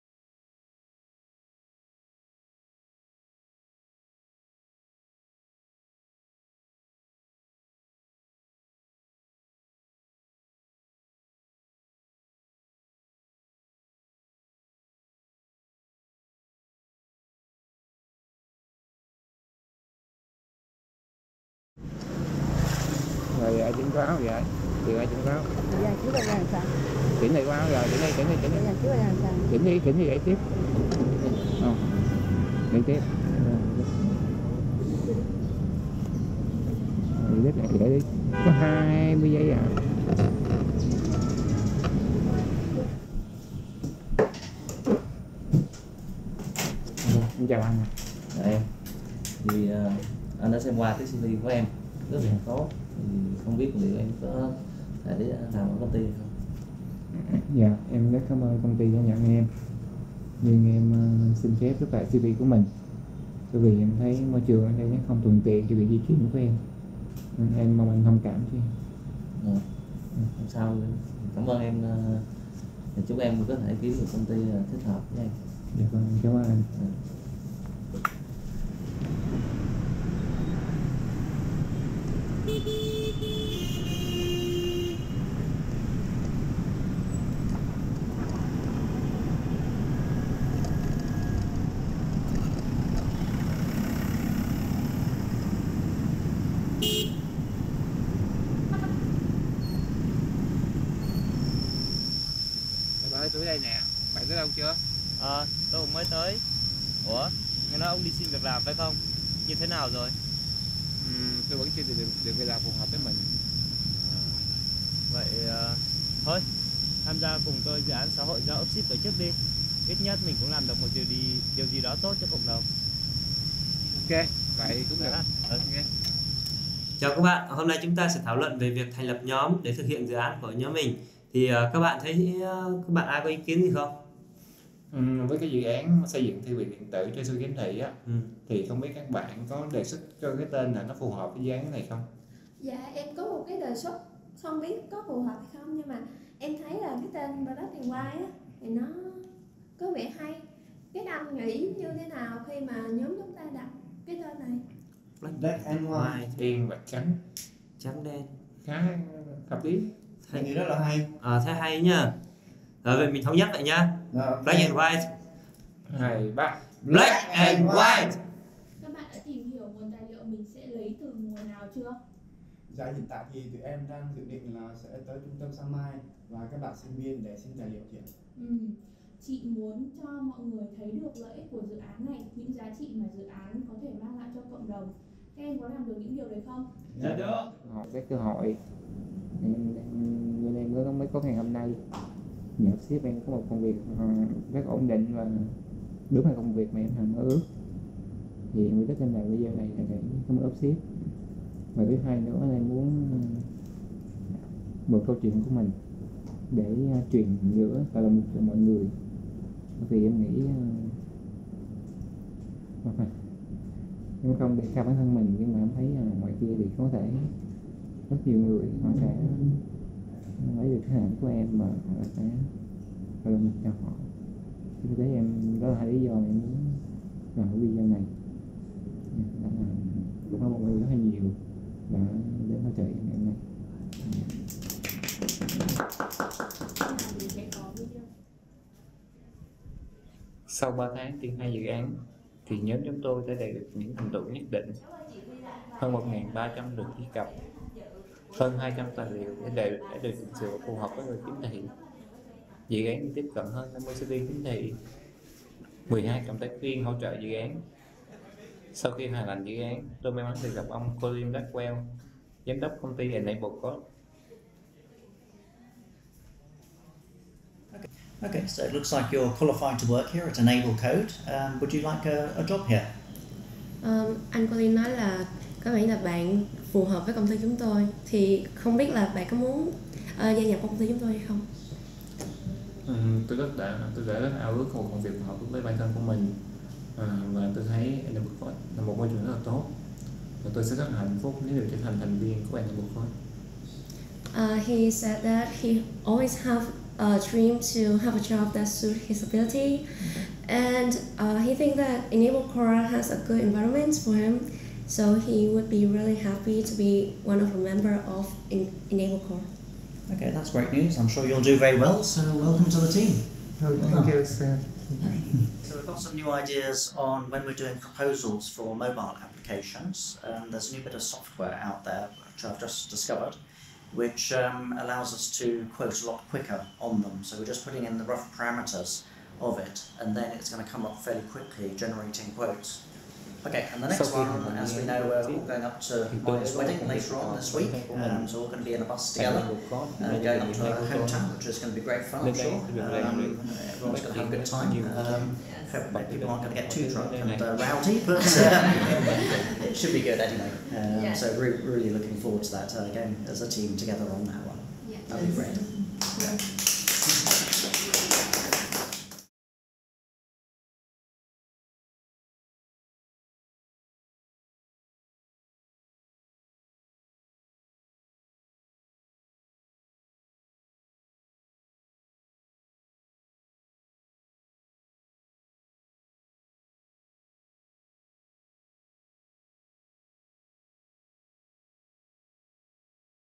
My-World-My-Life-My-Feet-Enablecode-Vietnamese
UNICEF's UPSHIFT is a youth-led program which empowers young people with skills, opportunities, and mentorship to innovate and bring their social impact projects to life. In many countries including Vietnam UPSHIFT has helped young people to raise their voice, start up their own business or get a job.
UPSHIFT has helped develop personal empowerment and employability skills for young people like Nguyen Toan. After incubating his project with UPSHIFT he was offered a job with Enablecode, an HCMC based IT company employing young people with disabilities.